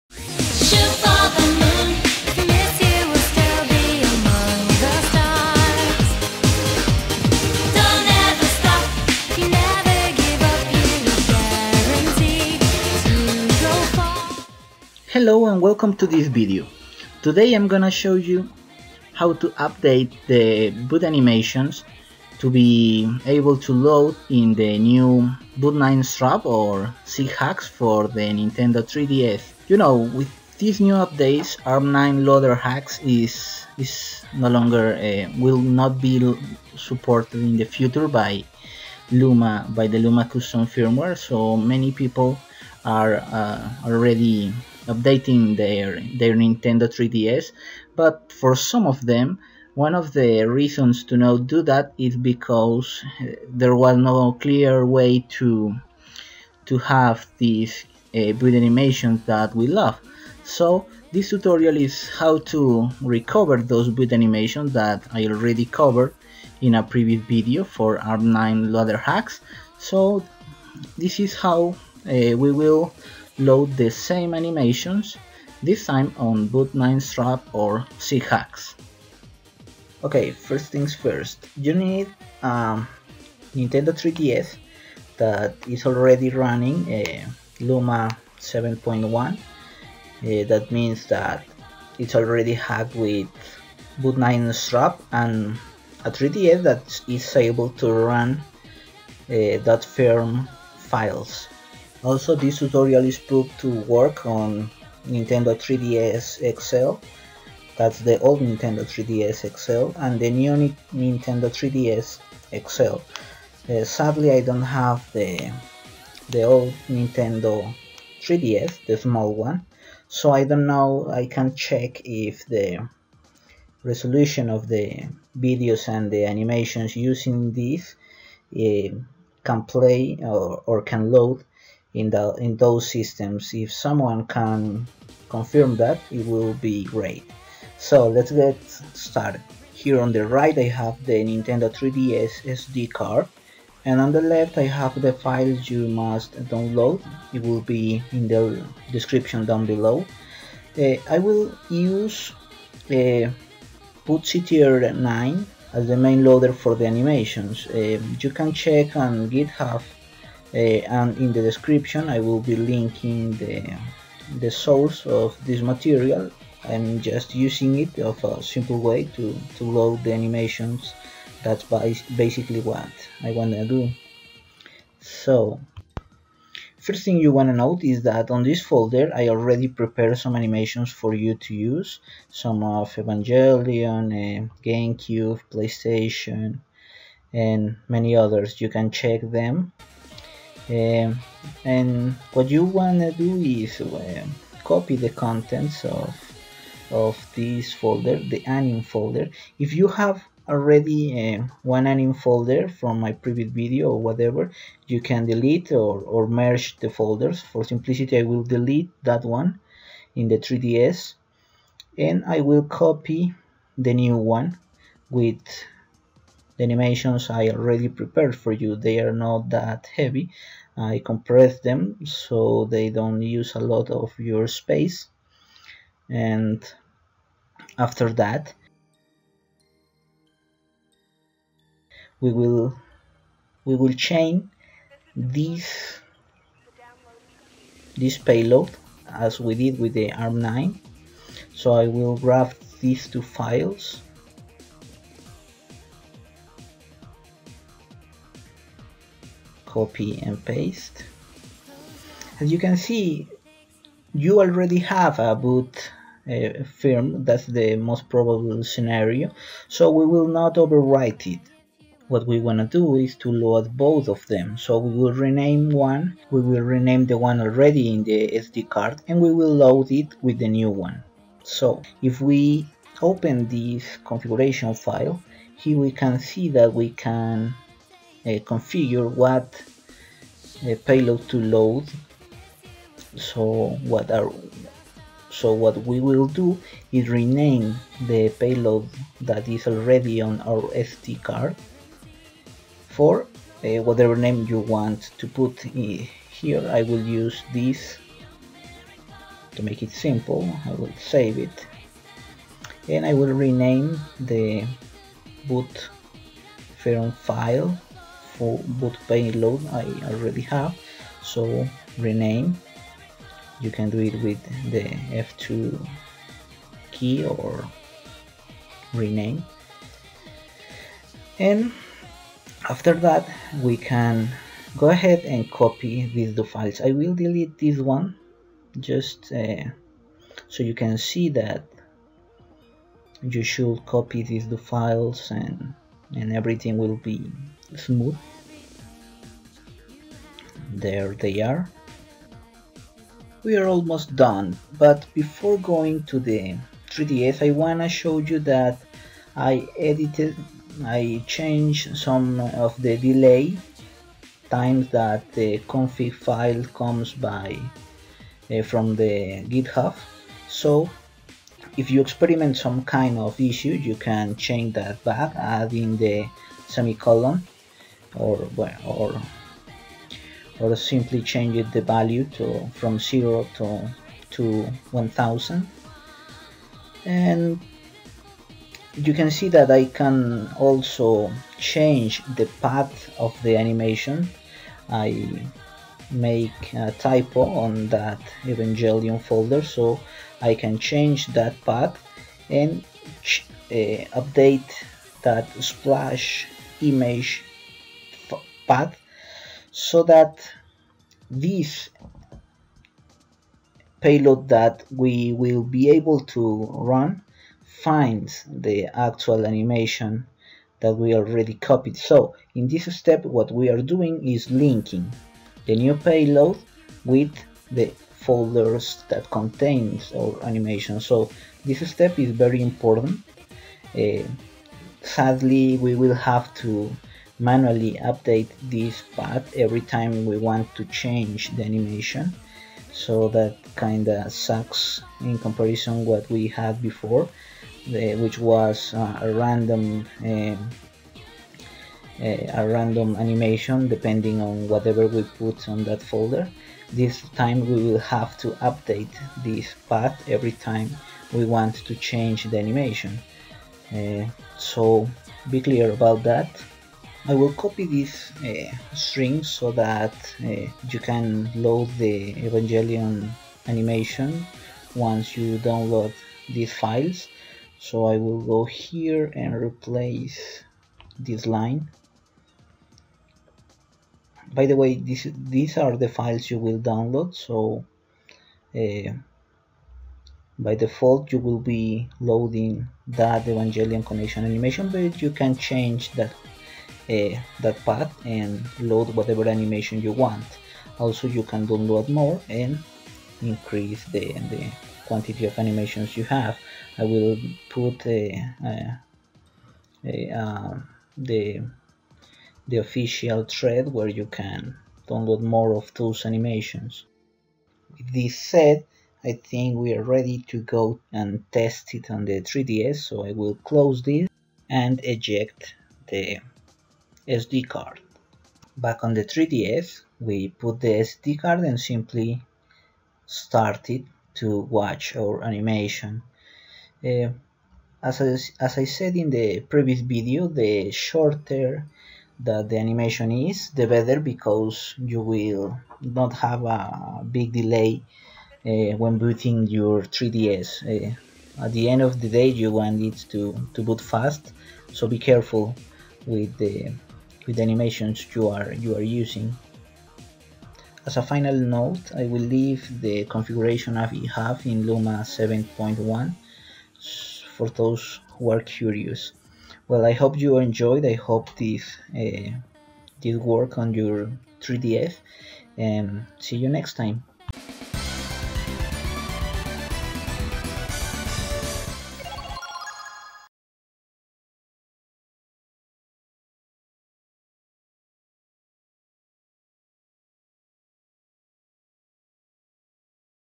Boot9Strap-SIGHAX Update - Animated Boot Screen for Nintendo 3DS/ luma7.1 and 8.0
In this tutorial I cover the essentials loading old custom animations from Arm9LoaderHax using the new Boot9Strap, also known as Sighax. I already covered how to make these animations, so if you wanna check on that, feel free to go here:

Requirements for this tutorial:
Nintendo 3DS XL
Luma Custom firmware.
Boot9Strap, also known as Sighax
Access to the recovery menu (select + power button).
Time and passion for custom stuff-

Screen resolutions used:
TopScreen:        400 X 240 pixels
BottomScreen: 320 X 240 pixels
Tested on new Nintendo 3DS XL and Nintendo 3DS XL

If you love customizing your stuff as I do, this is a must have !!!. To aid you in that task, you can download all the resources used in this video on the following link.

For luma 8.0 you need a newer version of bootctr9.. B9s 1.2 compatible release:

Credits to all people in this threads who created some of the animations shown in this video:
The BootCTR9 release thread (main payload launcher we used)